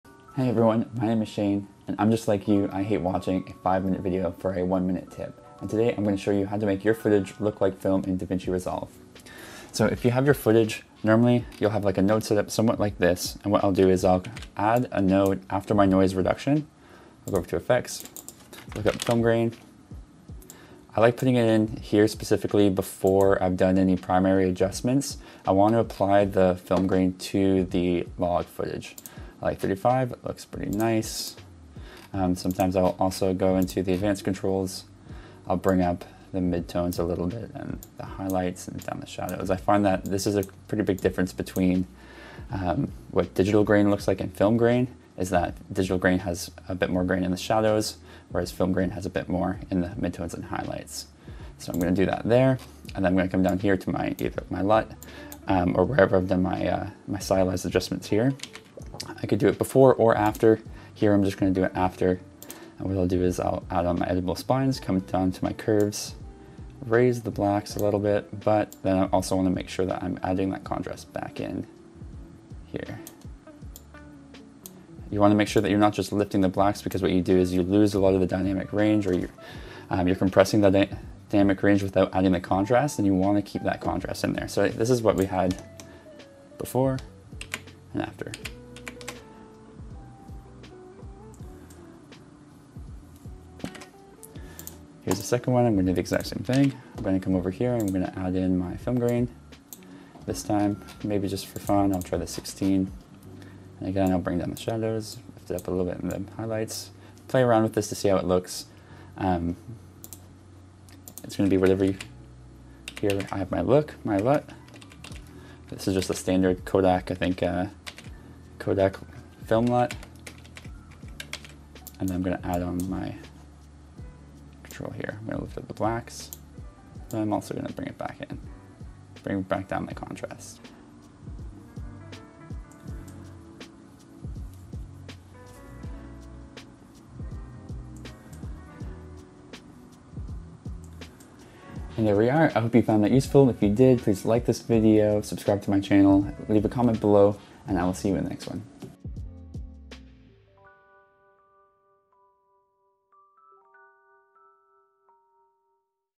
Fast & Easy FILM LOOK - Cinematic Footage in 1 Minute
Davinci Resolve 18 tutorial. How to get the cinematic film look on your footage in 1 minute. This is the best way that I've found to add grain and lift the shadows on your footage in DaVinci Resolve. There are so many different styles of film stock, and the colours will vary depending on the stock you want to emulate but I've found that all variations of the Film look have these features in common!

CANON R7
Variable ND Mount